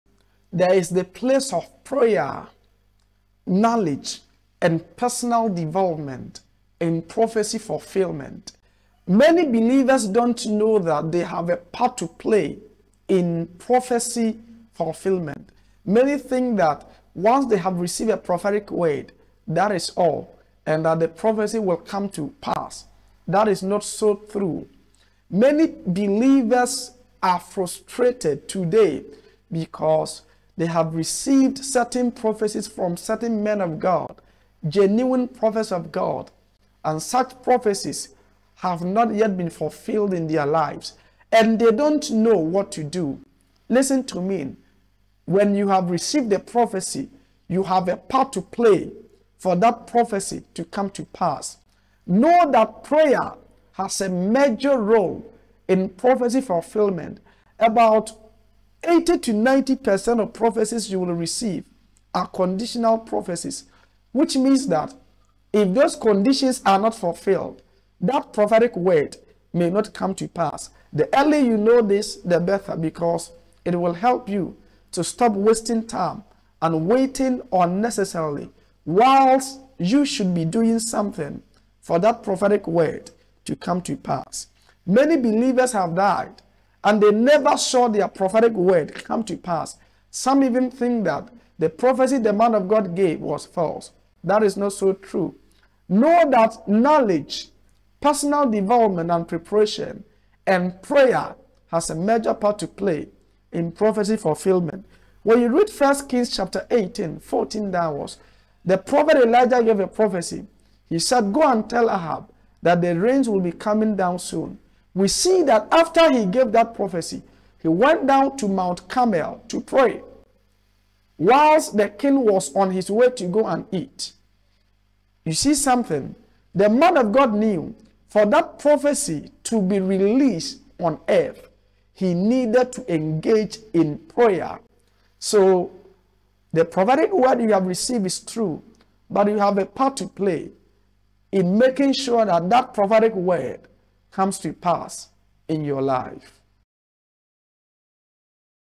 YOUR PART TO PLAY IN PROPHECY FULFILLMENT
What you are supposed to do to make the prophecy on your life come to pass